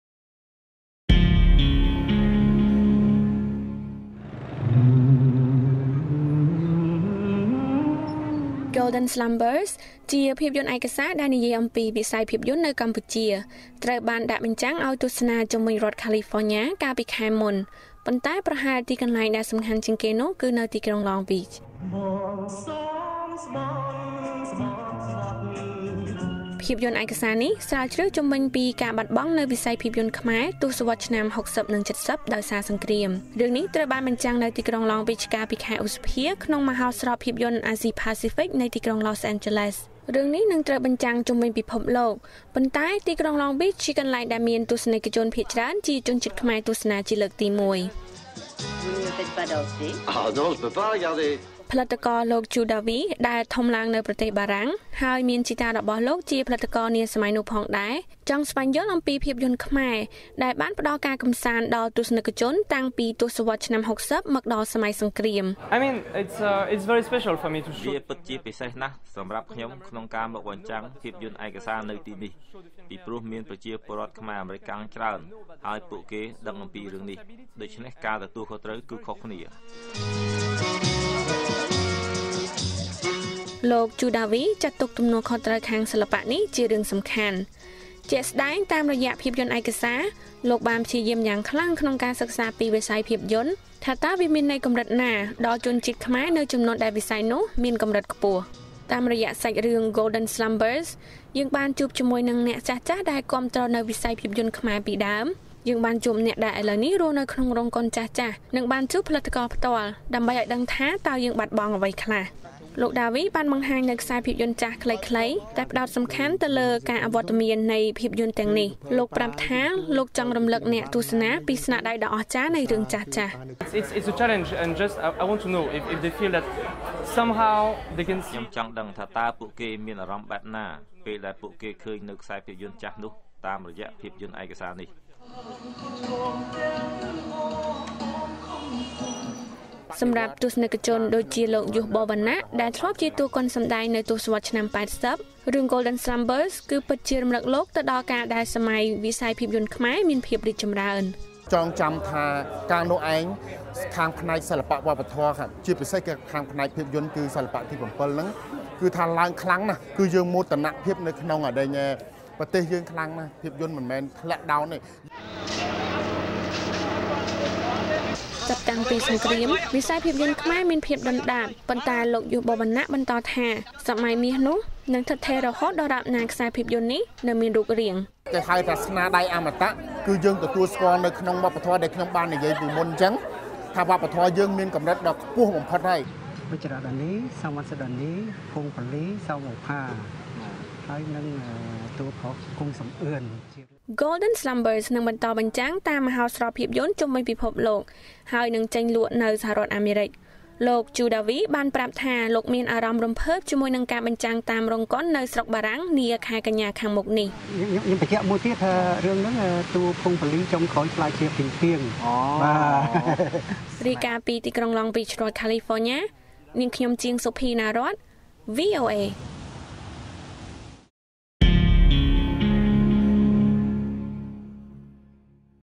Cinema Documentary Finds Fans in Long Beach (Cambodia news in Khmer)
"Golden Slumbers," a much-anticipated documentary on Cambodian cinema, explores the loss of the country's film industry to war. It played in Long Beach in May as part of the Los Angeles Asian Pacific Film Festival. Producer Chou Davy, who grew up in France and whose grandfather was a producer of that era, wanted to learn more about the movies that kept so many Cambodians entertained through the 1960s and into the war years. He says the Long Beach screening was a major test of the film's authenticity. VOA Khmer's Cheang Sophinarath reports.